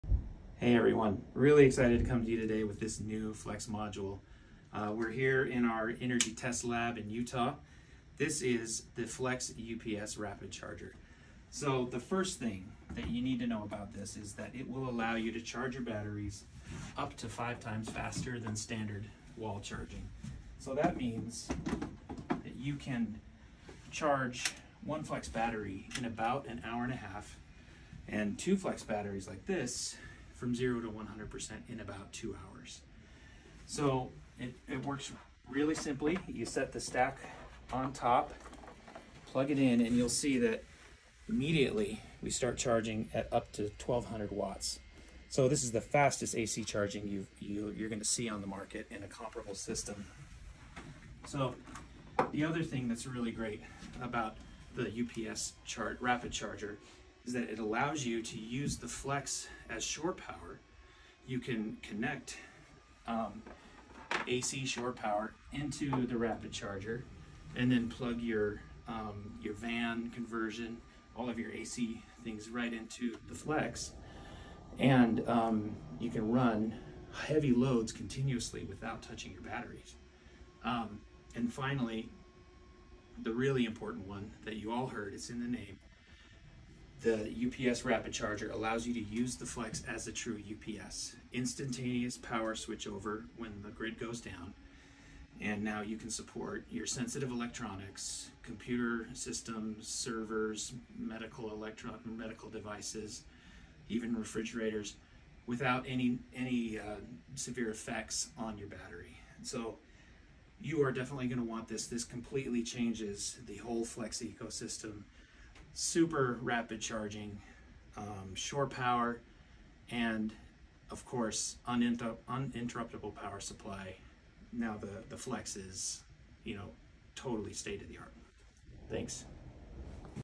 The Flex UPS Rapid Charger Totally Changes the Solar Generator Game
Looking for a fast and reliable way to charge your Flex portable solar power system? Look no further than the Flex UPS Rapid Charger! This innovative device allows you to charge your Flex system at speeds of up to 1200 watts of AC, so you can be sure your batteries are ready when you need them.

Not only does the UPS Rapid Charger provide lightning-fast charging, it also allows you to use your Flex system as a true UPS. With the ability to protect loads up to 1200W when plugged into shore power at the campground or the wall outlet at home, the UPS Rapid Charger ensures that your critical devices keep running even when the grid goes down.

Thanks to the UPS Rapid Charger, you'll never have to worry about running out of power when you need it most. Whether you're on the go or at home, you can rest easy knowing that your important devices are in good hands.

So why wait? Add the UPS Rapid Charger to your Flex stack today and start enjoying all the benefits of fast, reliable charging and automatic power backup!